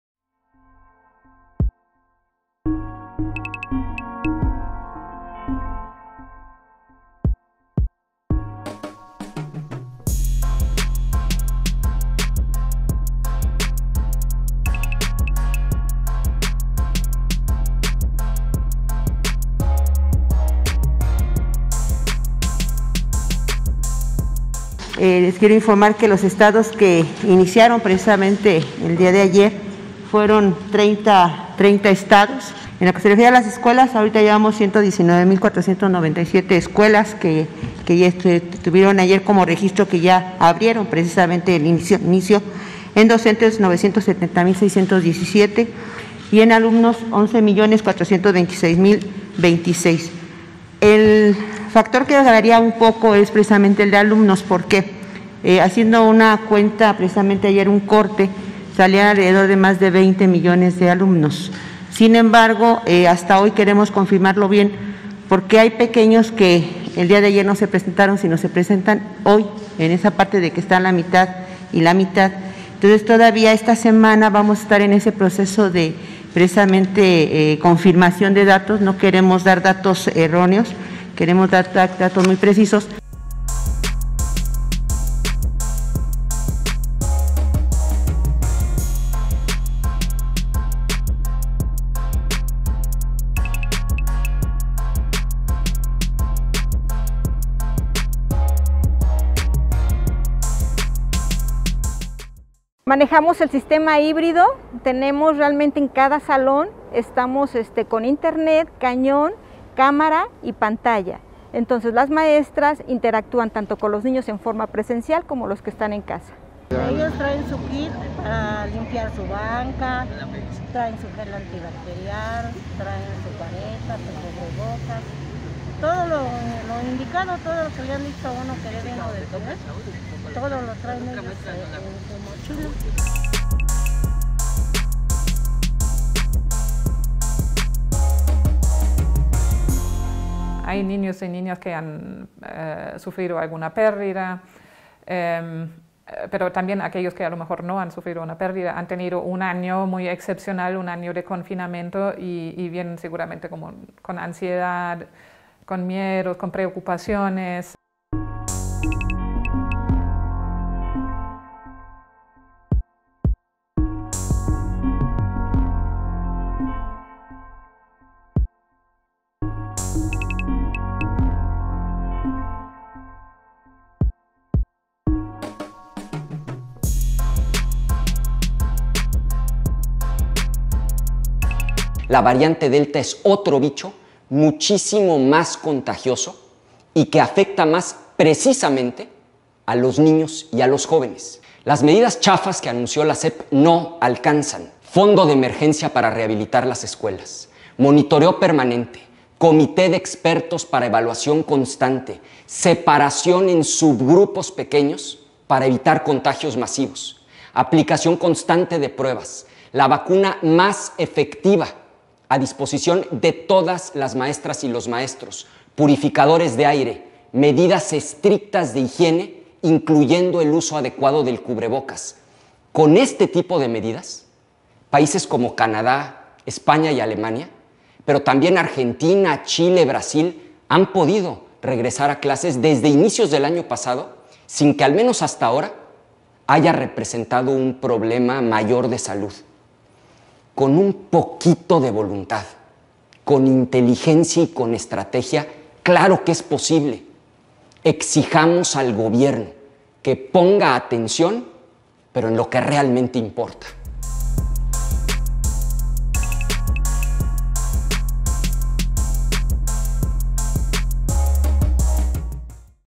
11 millones de alumnos volvieron a clases
¿El regreso a clases presenciales se verá afectado por los contagios de covid-19?

La secretaria de Educación Pública, Delfina Gómez Álvarez, informó que unos 26 millones de alumnos de nivel básico regresaron a clases presenciales, aproximadamente 11 millones 426 mil 26 en 119 mil 497 escuelas de 30 estados del país.